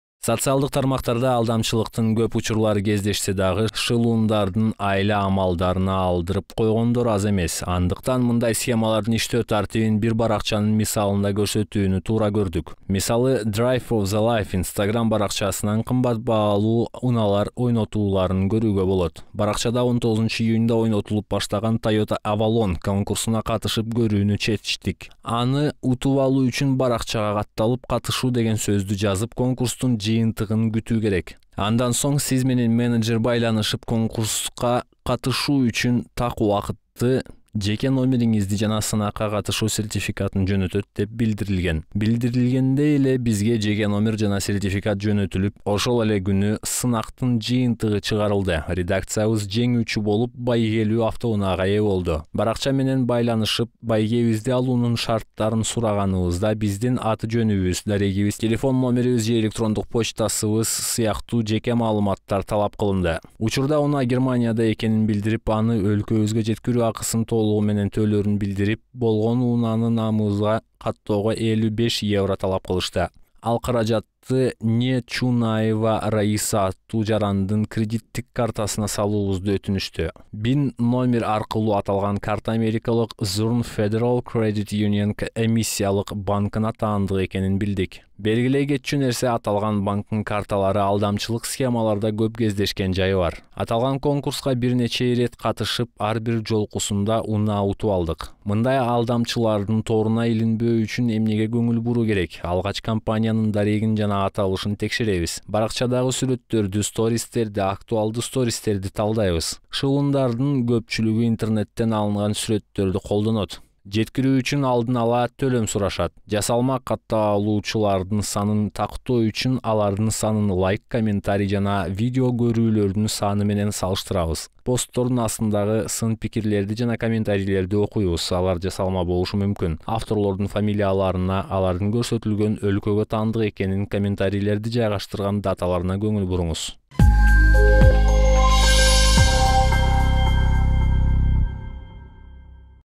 ИНТЕРНЕТтеги АЛДАМЧЫЛАРдан кантип САК БОЛУУ керек
🔷 Соцтармактарда фейк белектердин ойнотулушу, "комментарий жазгандарга акча берем" деген убадалар сейрек эмес, бирок ага карабай адамдар дагы эле шылуундардын тузактарына илинип калышууда.
🔷 FactCheck KG бир ушундай мисалдын негизинде, шылуундардын схемасы кандай иштээрин көрсөтүп берет.